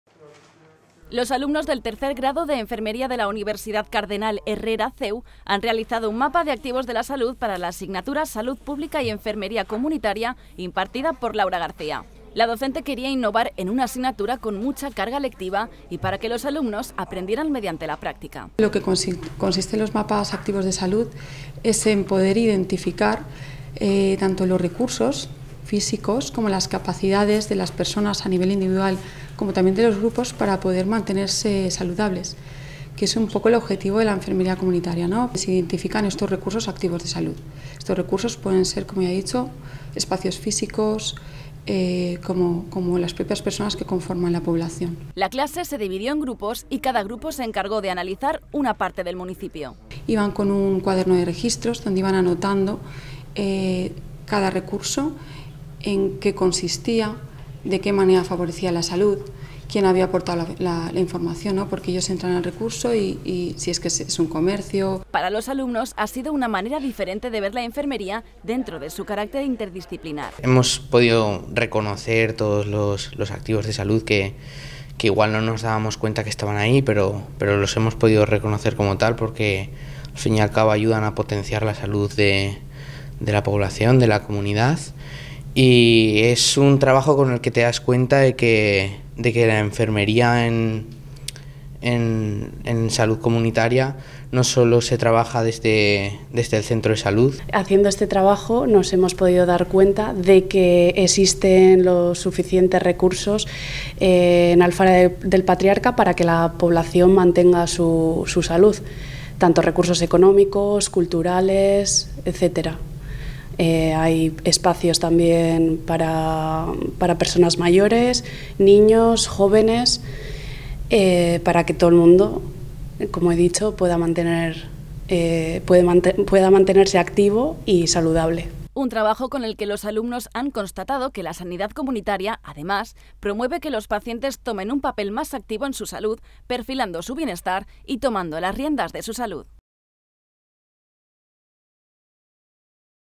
Los alumnos de 3º de la UCH- CEU analizan la salud de Alfara del Patriarca | Cecova TV
Los alumnos de 3º del Grado de Enfermería de la Universidad Cardenal CEU han realizado un mapa de activos de la salud para la asignatura Salud Pública y Enfermería Comunitaria impartida por Laura García. La docente quería innovar en una asignatura con mucha carga lectiva y para que los alumnos aprendieran mediante la práctica.